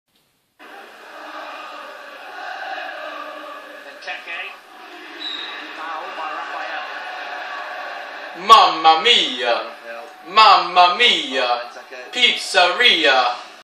Rafael swears into TV camera
Rafael caught swearing into the TV camera at the Aston Villa vs Manchester United match. Must watch funny.